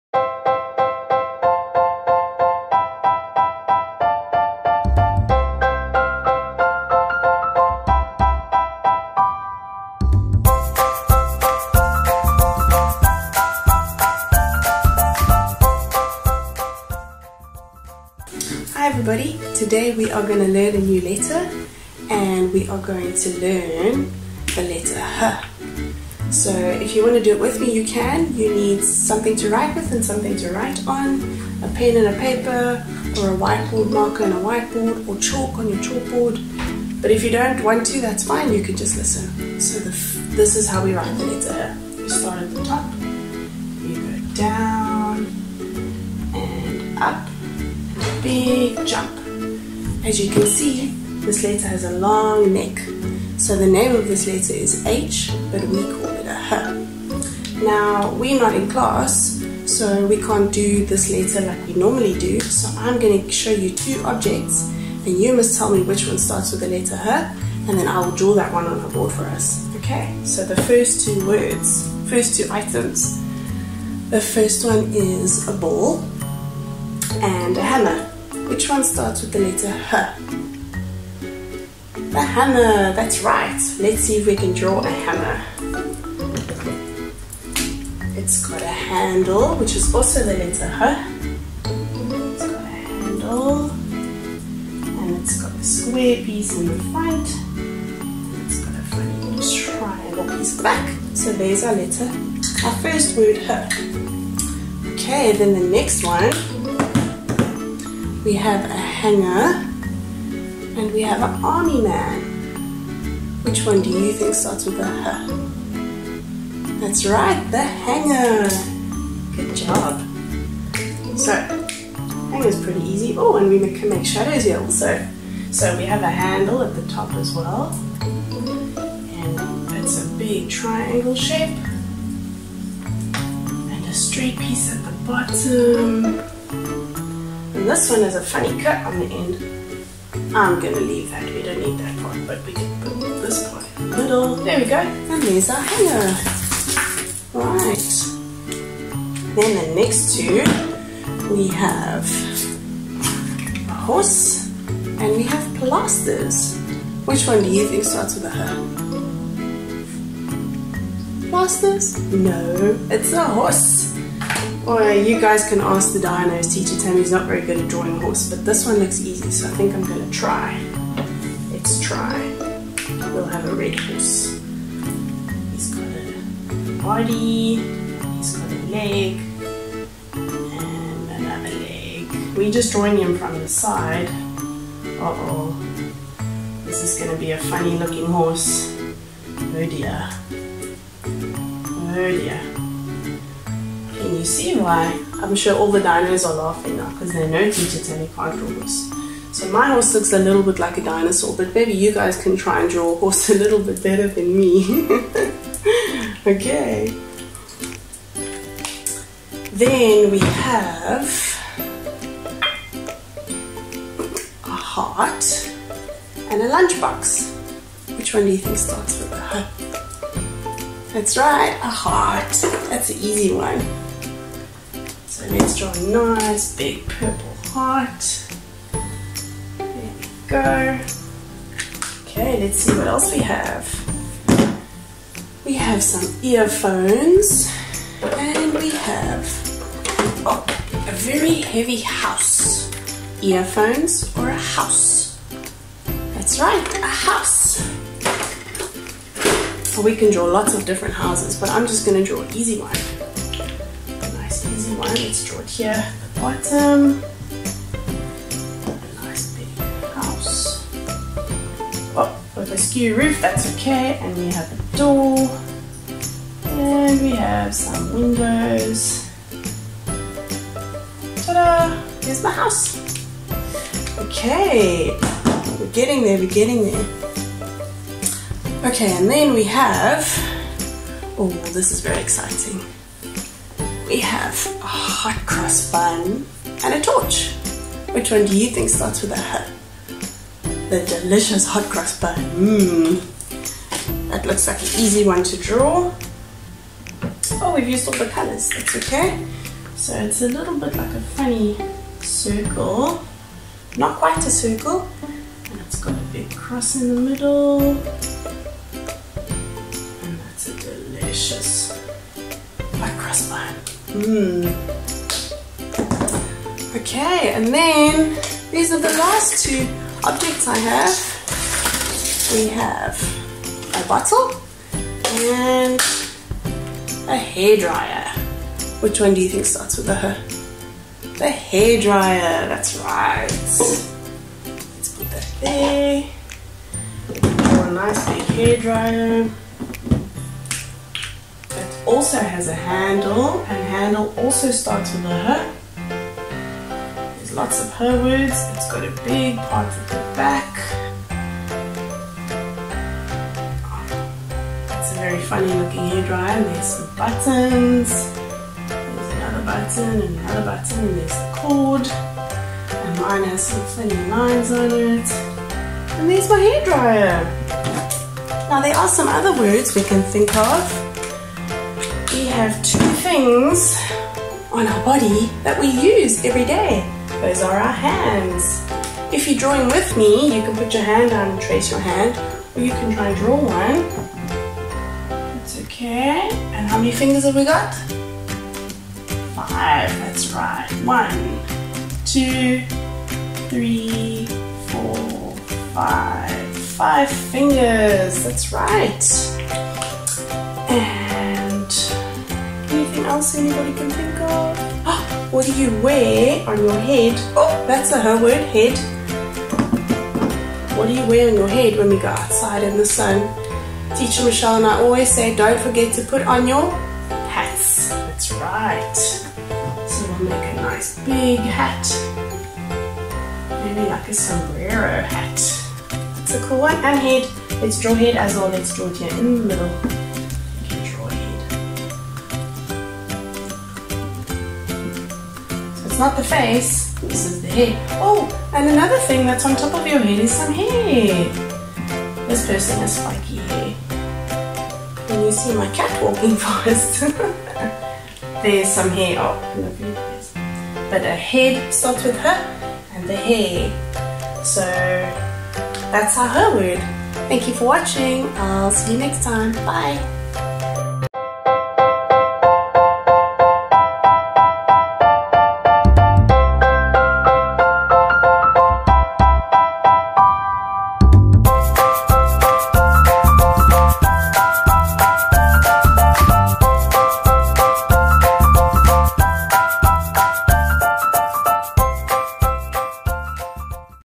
Teacher Tammy Learn your letters letter h
Teacher Tammy shows us how to draw the letter h, how to pronounce it and what things in your house start with the letter h.